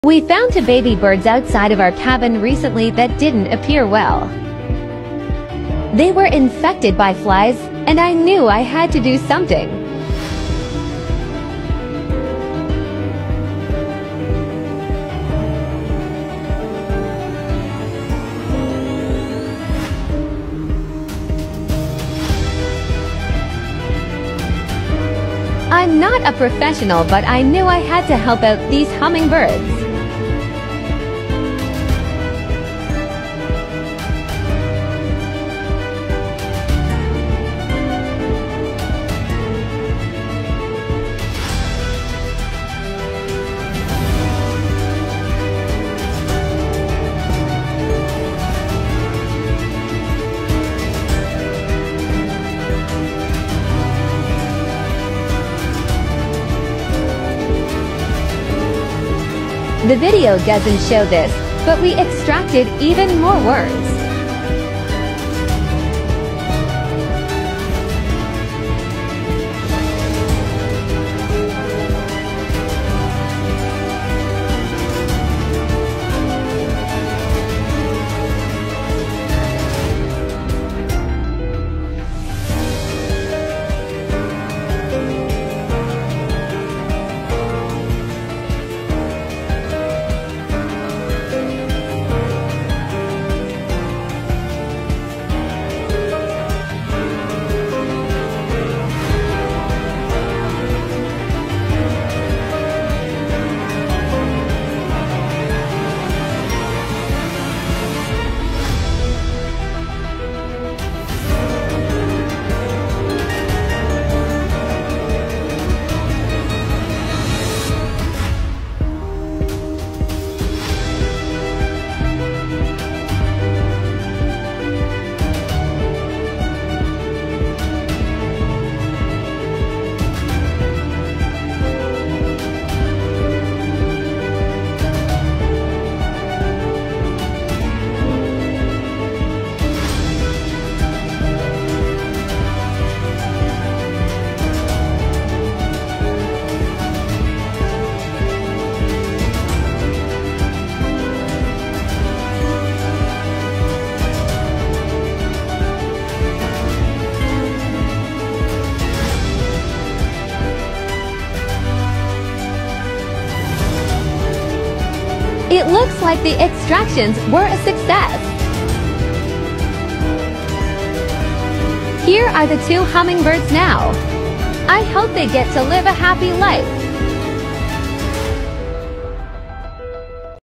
Botflies Extracted From Baby Hummingbirds
Resilience in Nature: Witness the heartwarming journey of a baby hummingbird as it triumphs over adversity. 🌟 After being infested with botflies, this tiny avian warrior underwent a successful extraction process. 🐦💪 Now, it soars through the skies once more, reminding us of the power of healing and survival. NatureResilience HealingJourney HummingbirdComeback TriumphOverAdversity"